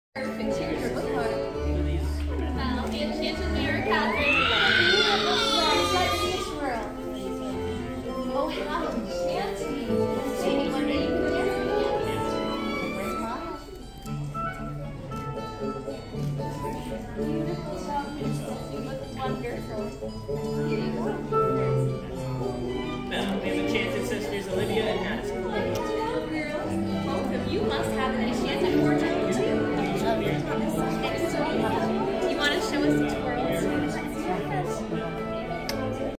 Catherine meeting Belle, twirling her dress
7 years old Disney March 2024